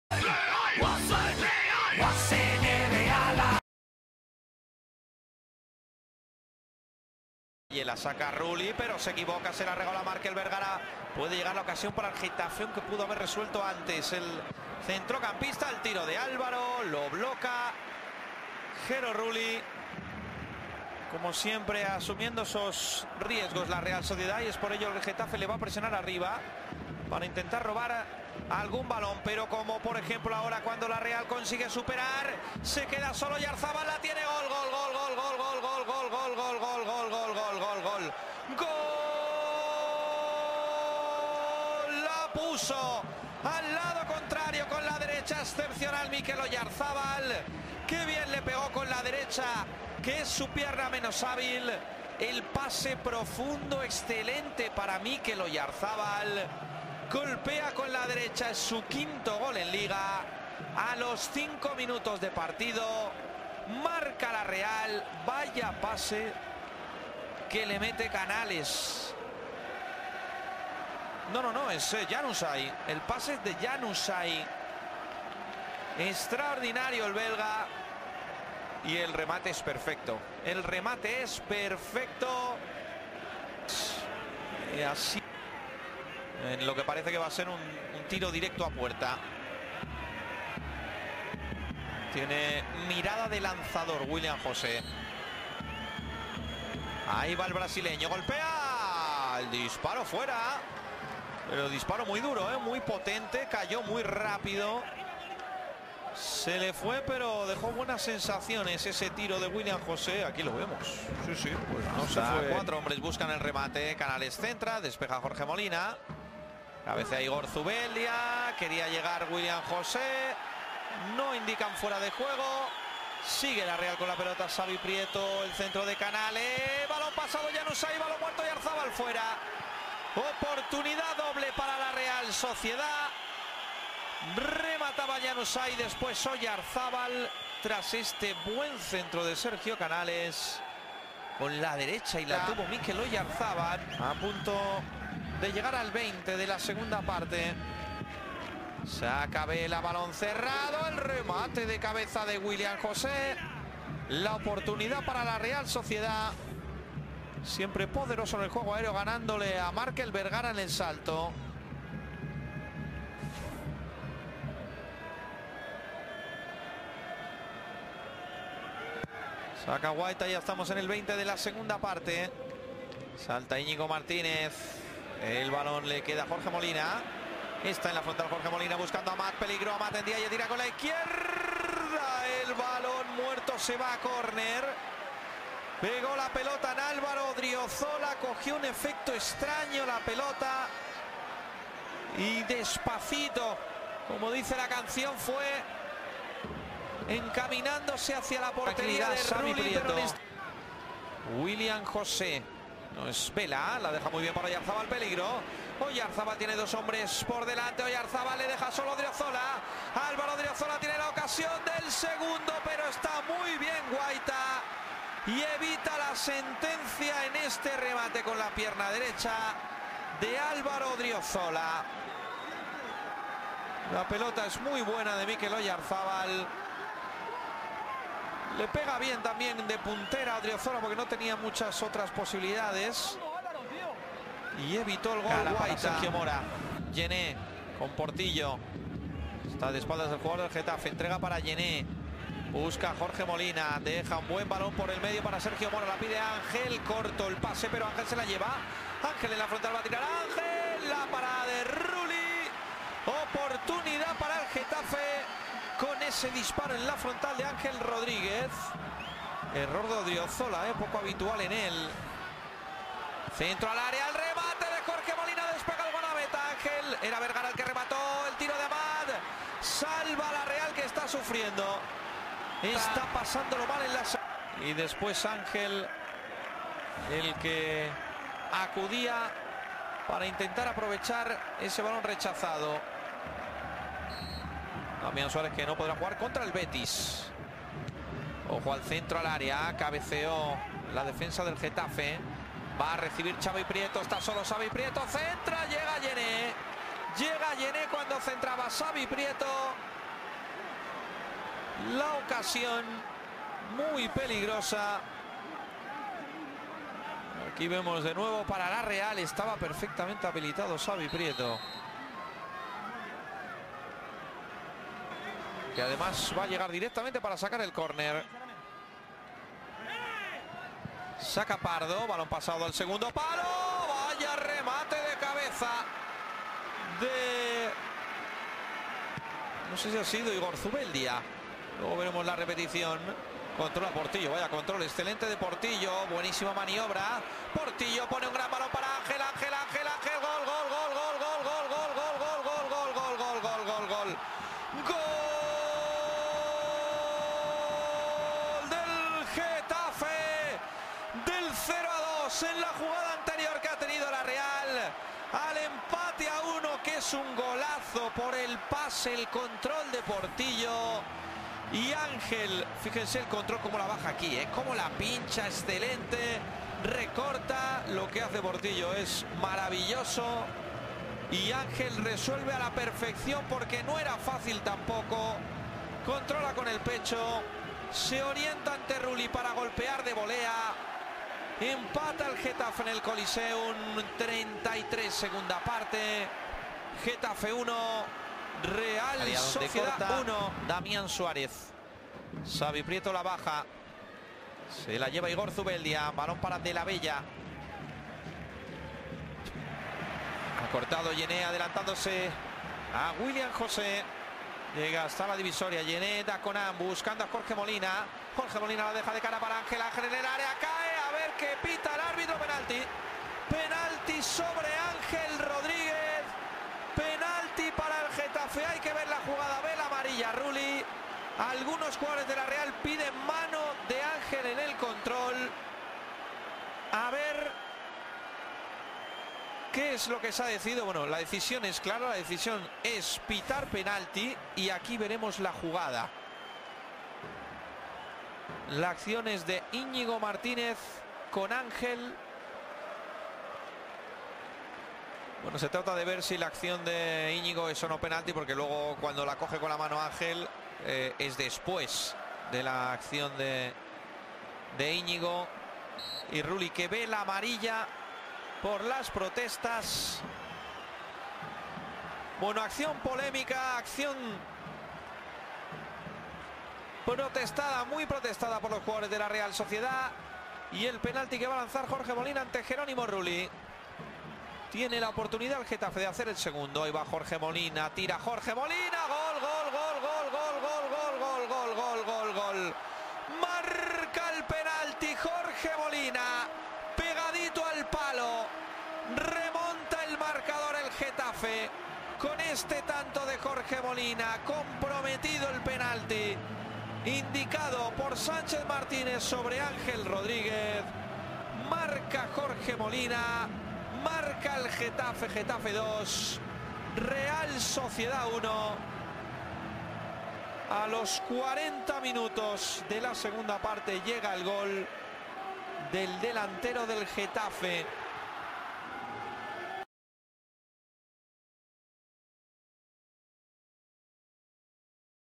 Getafe C.F. 2-1 Real Sociedad (Amplio Resumen) Jornada 10 LaLiga
Todas las jugadas más importantes y goles del partido en directo.

FICHA TÉCNICA

GETAFE, 2

Guaita, Antunes, Djene, Cala, Damián, Markel Bergara, Aranbarri (Mora, m. 69), Fajr (Ángel Rodríguez, m. 56), Amath, Álvaro (Portillo, m. 69) y Molina

REAL SOCIEDAD, 1

Rulli, Odriozola, Llorente (Aritz Elustondo, m. 67), Iñigo Martínez, De la Bella, Zubeldia, Canales (Pardo, m. 76), Prieto, Januzaj (Vela, m. 59), Oyarzabal y Willian José.

Goles: 0-1: Oyarzabal (m. 5). 1-1: Ángel (m. 77). 1-2: Jorge Molina, de penalti (m. 84)

Árbitro: Sánchez Martínez. Amonestó a De la Bella, Djene, Damián Suárez, Antunes e Iñigo Martínez.